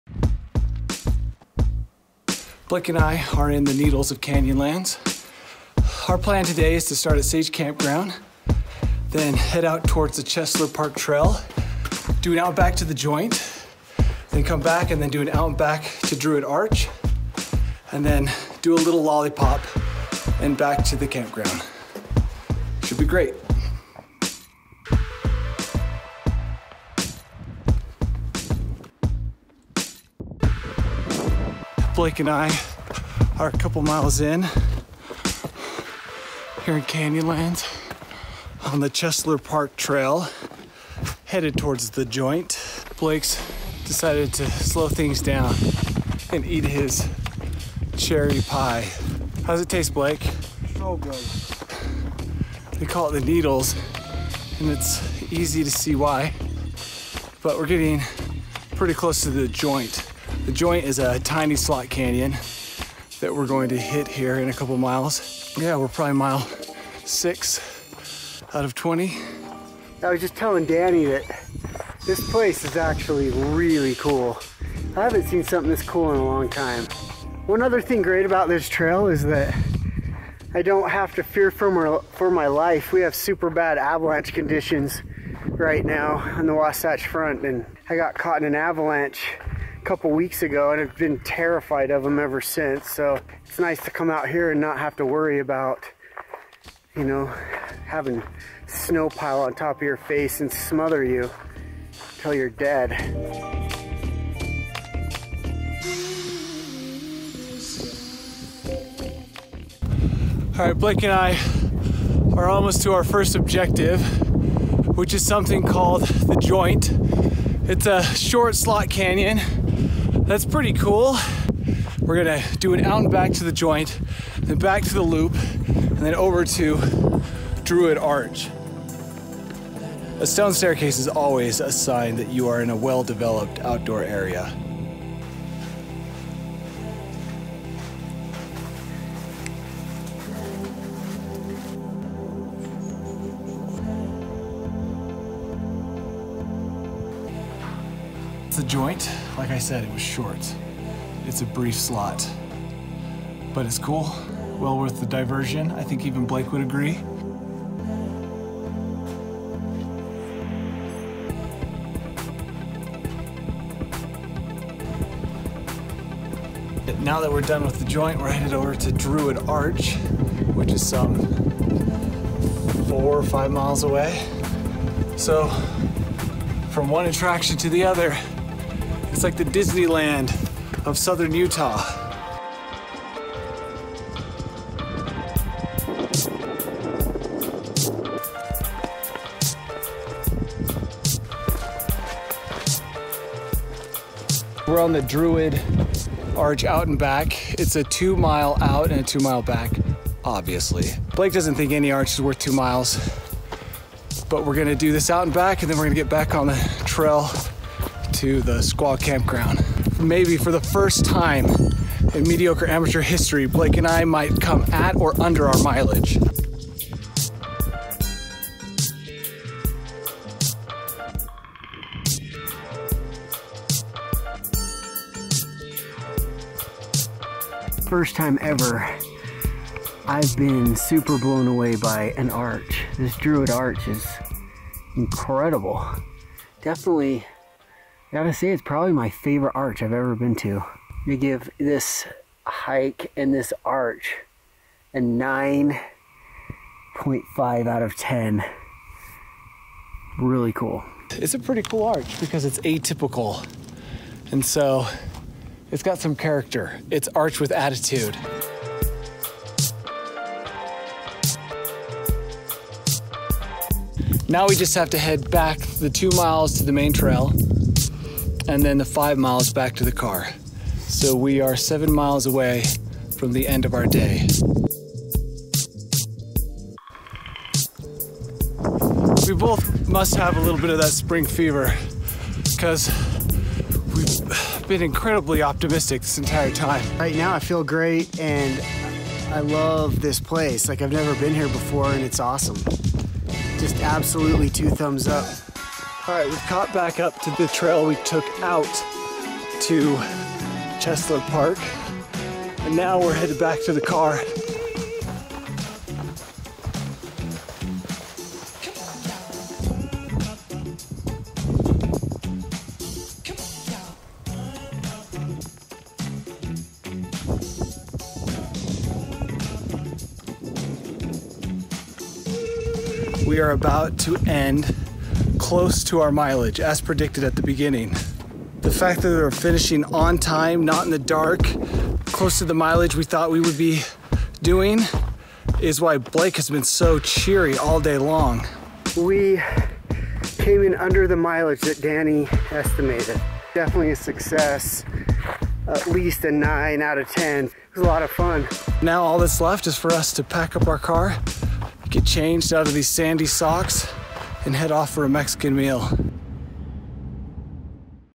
Canyonlands National Park: Blake Discovers a Natural Arch Worthy of His Praise!
Here's something you don't often see. An episode of Mediocre where Blake doesn't complain. It's like watching an episode of Seinfeild where Kramer doesn't come flying through Jerry's door. It's weird. Everything went right on our 19 mile run through the Canyonlands Needles District and the episode suffers as a result (I guess I'm complaining now). We hit all the hot spots like Druid arch, The Joint and Cheslar park. We also never got lost and managed to run only as many miles as we predicted. In other words, I think I may have been transported to another dimension where Blake doesn't complain and everything works out on our adventures. Which means you're from the same dimension and none of this makes sense.